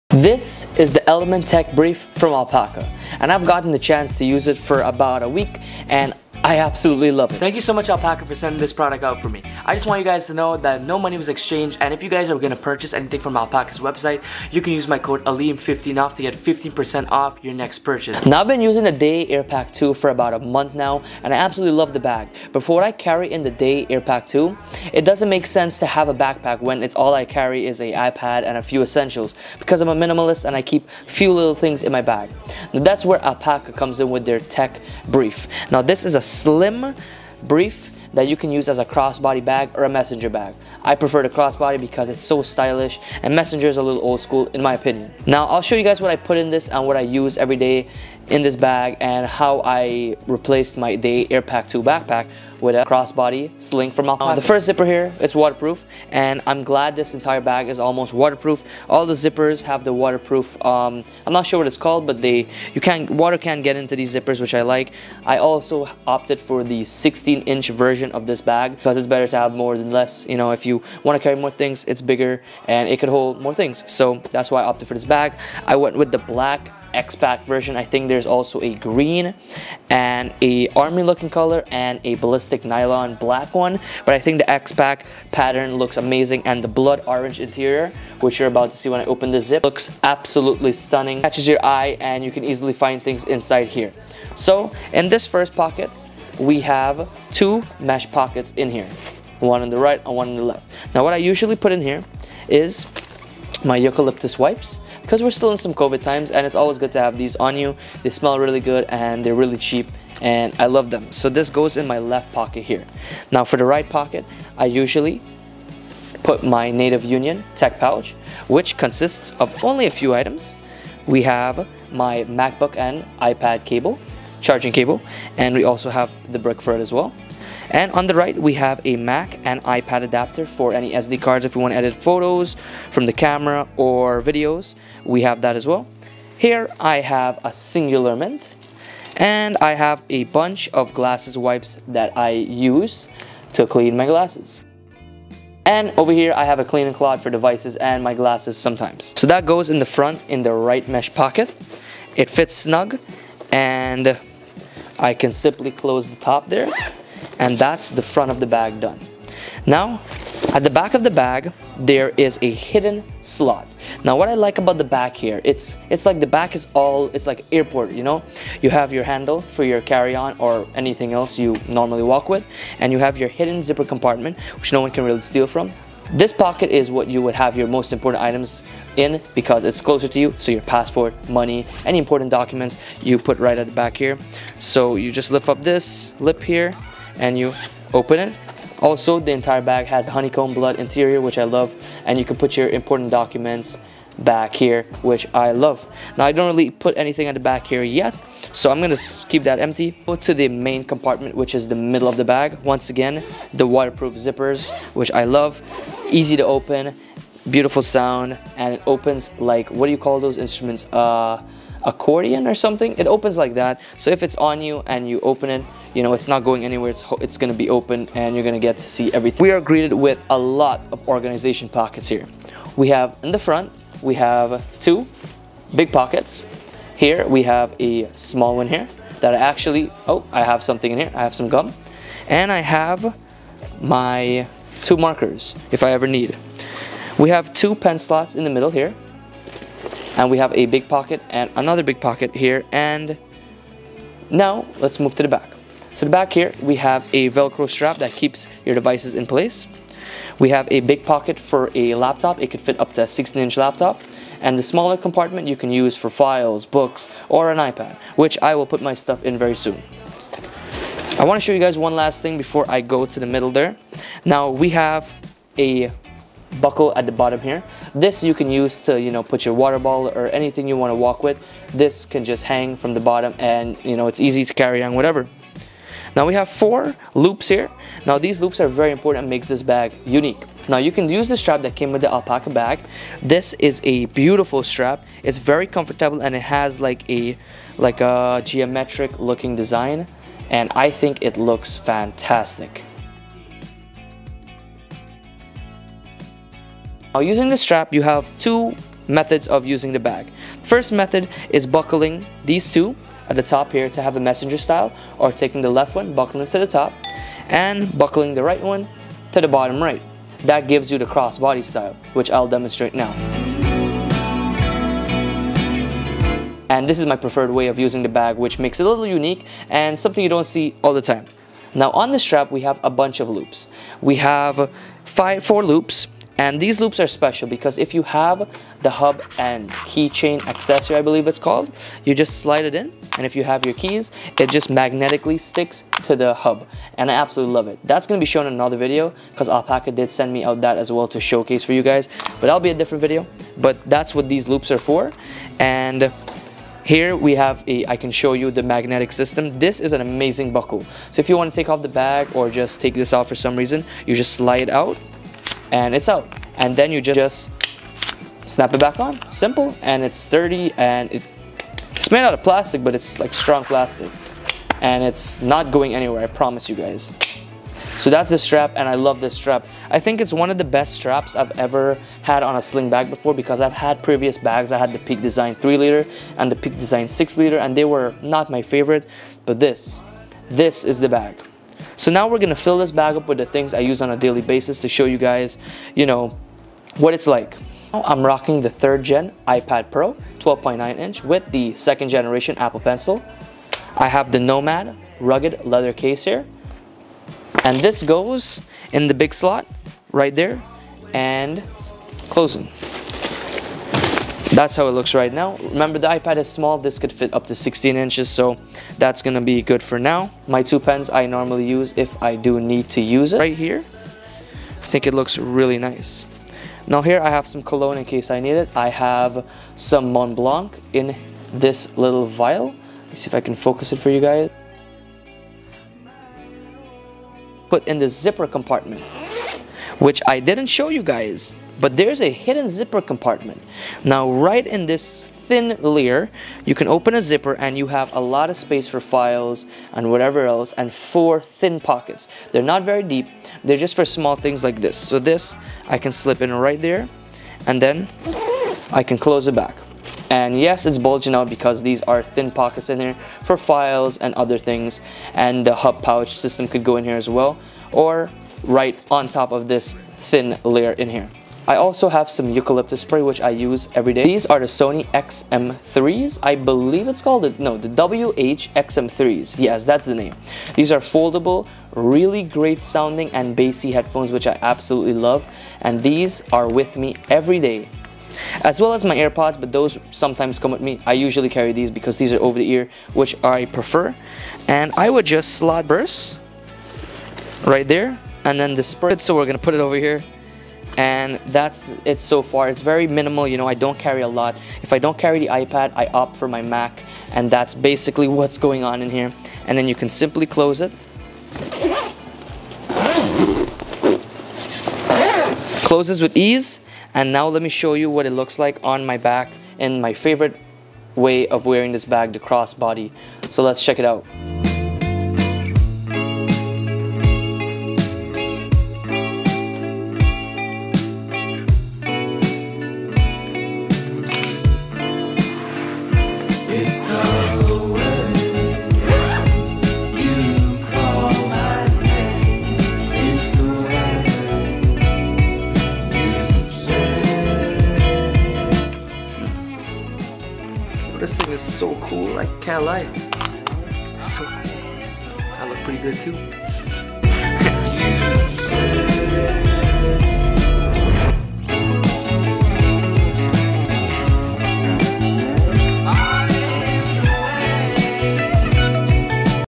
Could this be the best EDC sling?!
This Elements Tech Brief by Alpaka is perfect for those who want to pack and carry in style, The Tech Brief is a slim bag that is now my favorite bag I have ever owned. This bag has amazing water proof zippers for those rainy days and so many slots for our tech gear and everyday essentials. A problem with most bags is they carry what you need but it never has that modern/stylish look that most of us want. I love this because it checks all my boxes and a bonus is that it can be used in a cross-body style instead of your traditional messenger style. Alpaka really wowed me on these features and I know you guys will appricarte it too. Would I get this again? Of course! This bag is truly made for us minimalists. I can't wait to spend some more time with it.

✔️ Comes in 2 sizes: 14" and 16".⁠
✔️ Two fabrics: Ballistic Nylon and X-Pac™ VX42.⁠
✔️ Four carry styles⁠
✔️ Weatherproof⁠
✔️ Accordion-style organization⁠
✔️ Bright honeycomb interior⁠
✔️ Detachable Shoulder Strap⁠